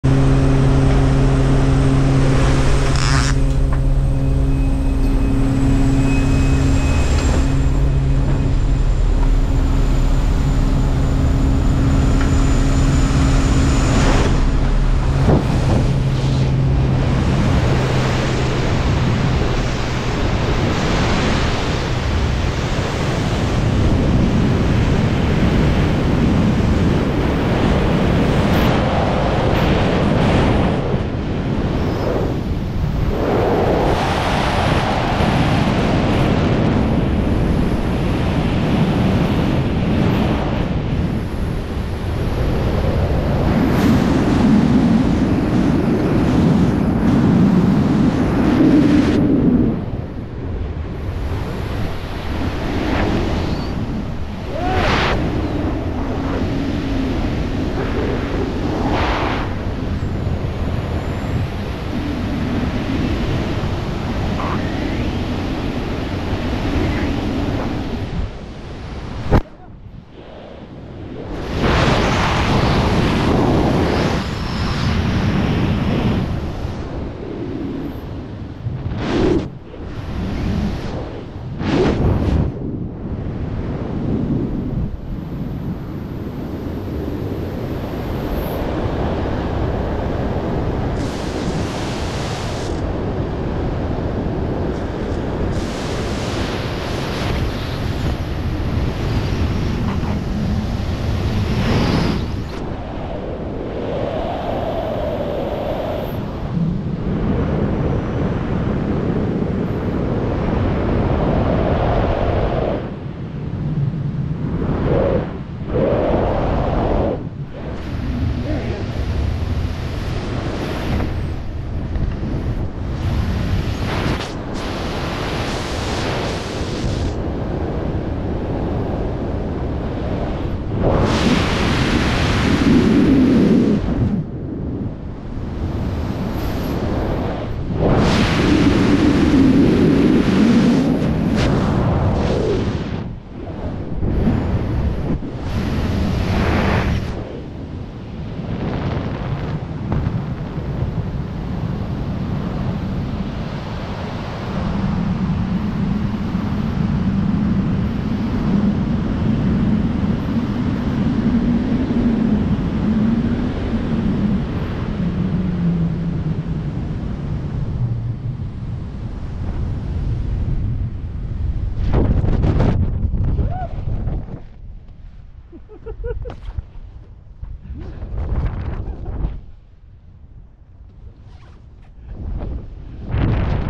Wingsuit fun over Eloy
Ryan making wingsuit dives a full contact sport. LOL